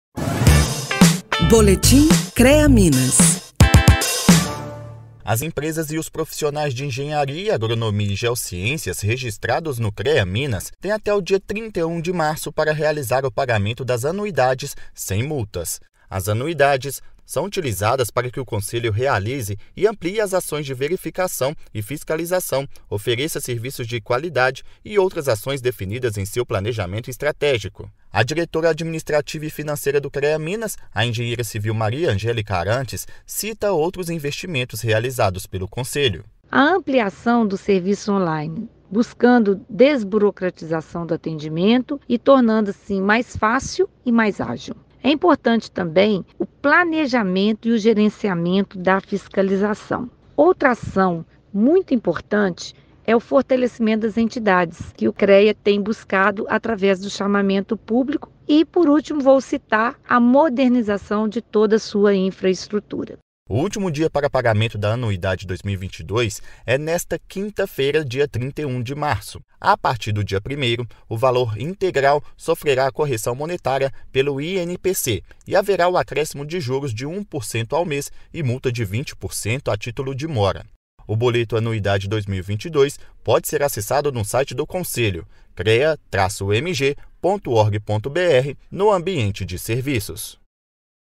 Termina, em 31 de março, o prazo para pagar a anuidade 2022 sem multa, juros e correção monetária
Profissionais e empresas de engenharia, agronomia e geociências têm até o dia 31 de março para pagar as anuidades de 2022 sem multas e outros encargos. Os recursos oriundos das anuidades são utilizados para que o Conselho fiscalize, ofereça seus serviços e realize outras ações estratégicas.

Confira a matéria da Rádio Crea-Minas.